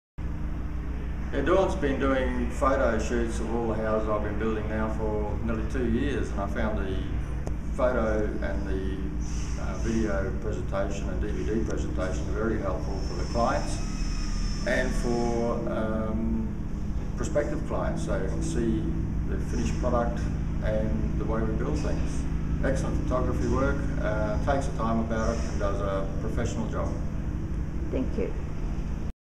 Dennis Field testimonial for Starsnap
Dennis Field, director of R&D Homes gives a testimonial for Dawn Smith of Starsnap Photography and Video. Dennis is a builder with vast experience and is highly regarded in the building industry. He realises the value of good house photography and the power that emotive video has in marketing.